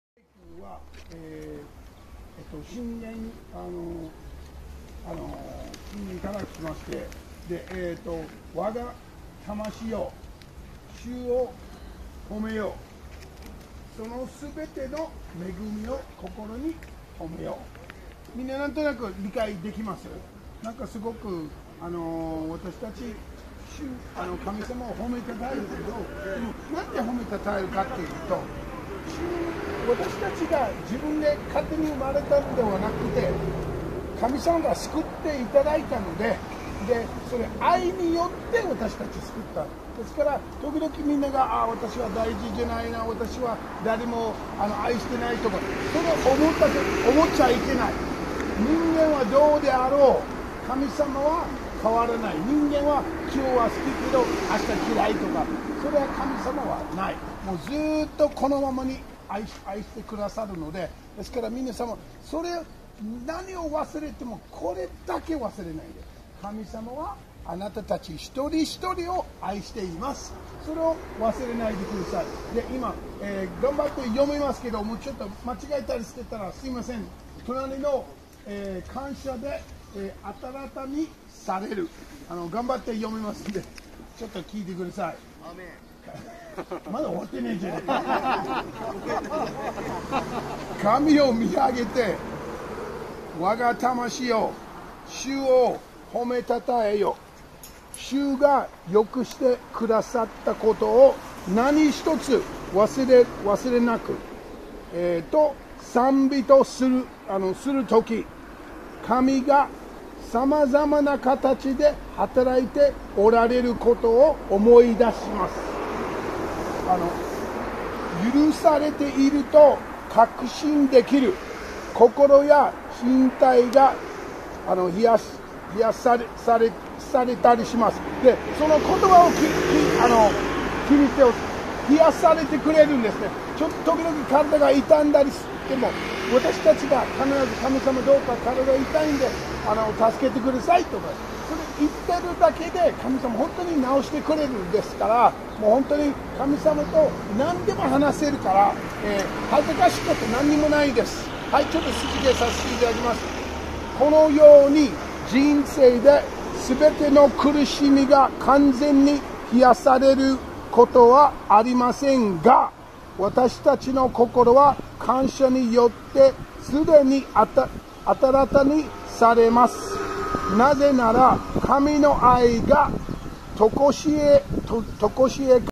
kindness to the Japanese homeless. 日本のホームレスへの優しさ
Brother Steve is sharing through Japanese how a message of hope to the homeless in Japan.